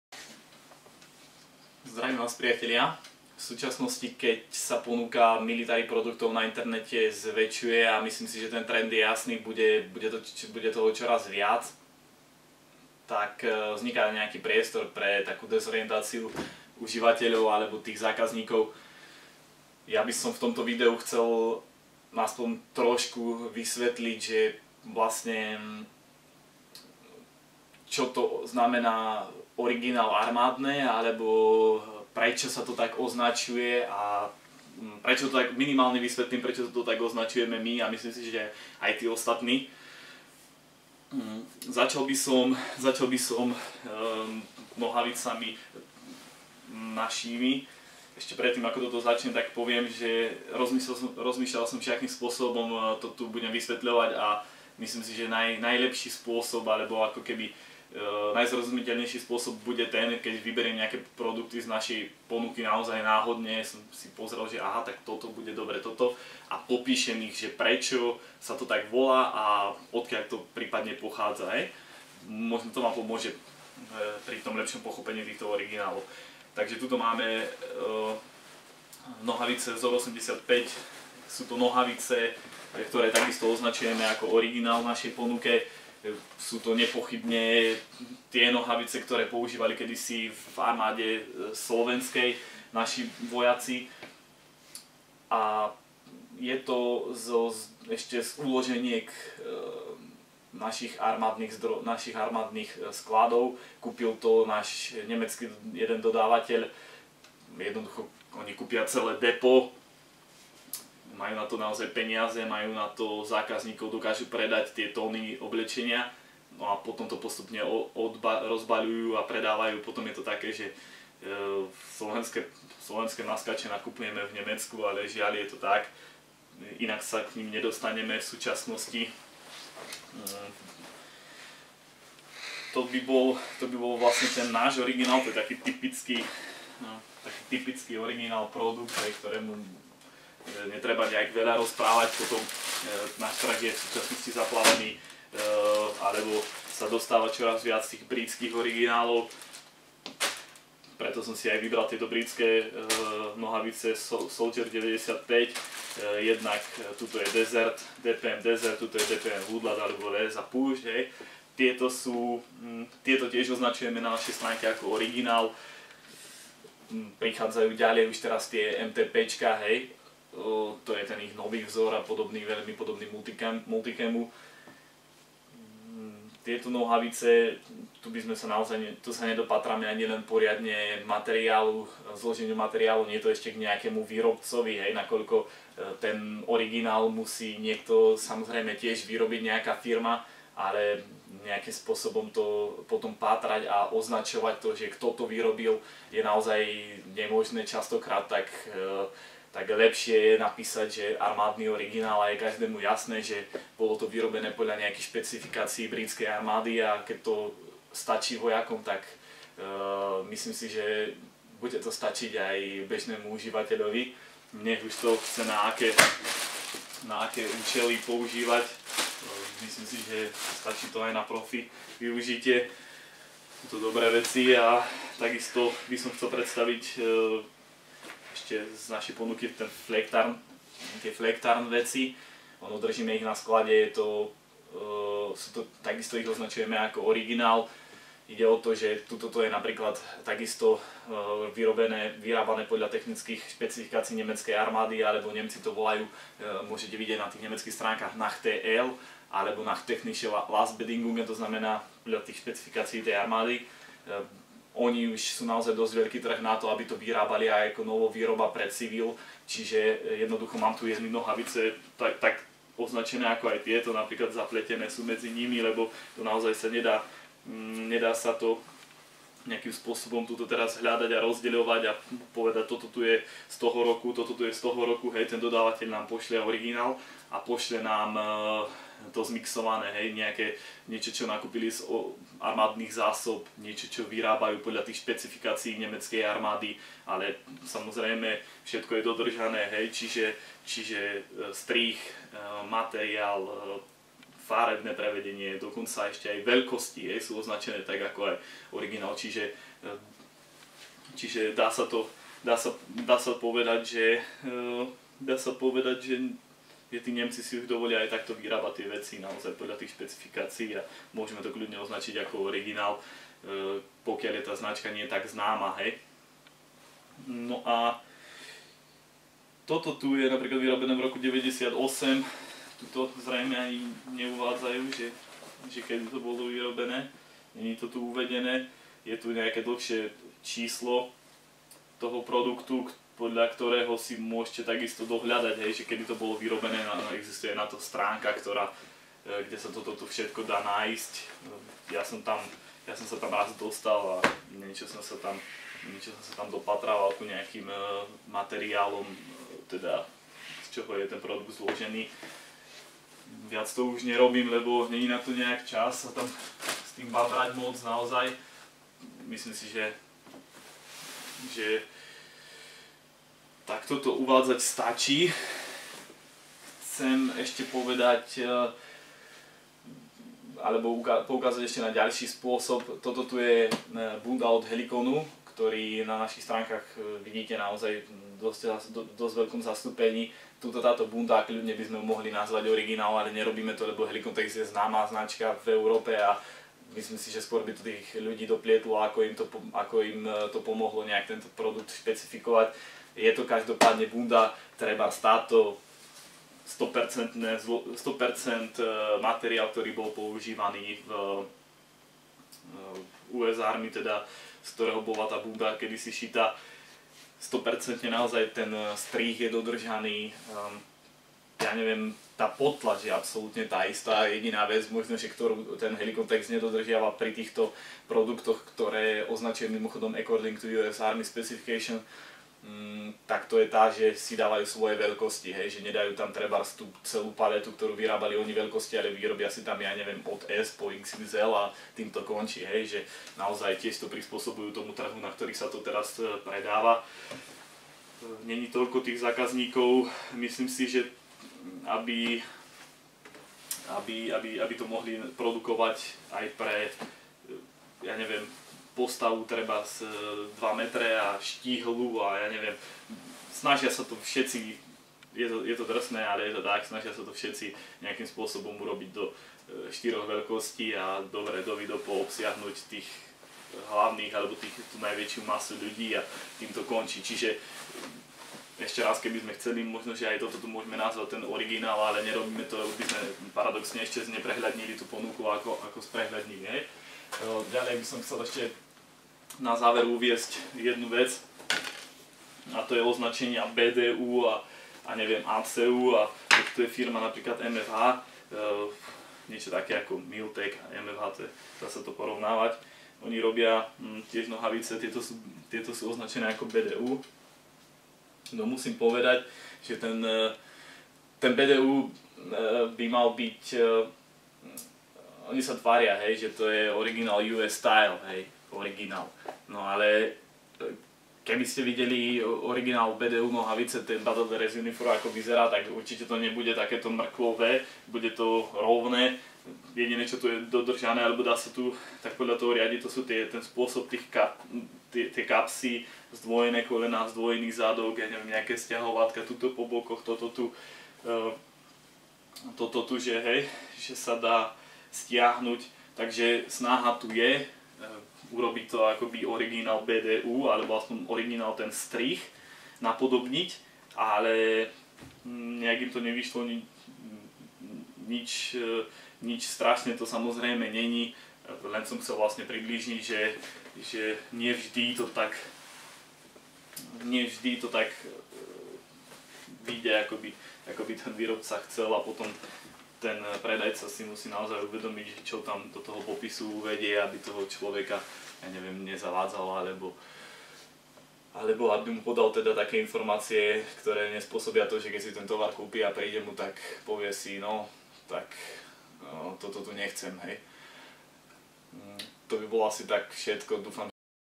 Originálne armádne oblečenie
V tomto videu by som chcel okomentovať význam slova "original" pri produktoch na našom webe Verím, že to bude pre niekoho prospešné a pomôže Vám lepšie pochopiť tieto armádne originály aj mimo stránok P.S: Plánoval som to "zmáknuť" lepšie, no dajte mi čas, ja to zlepším :) Marek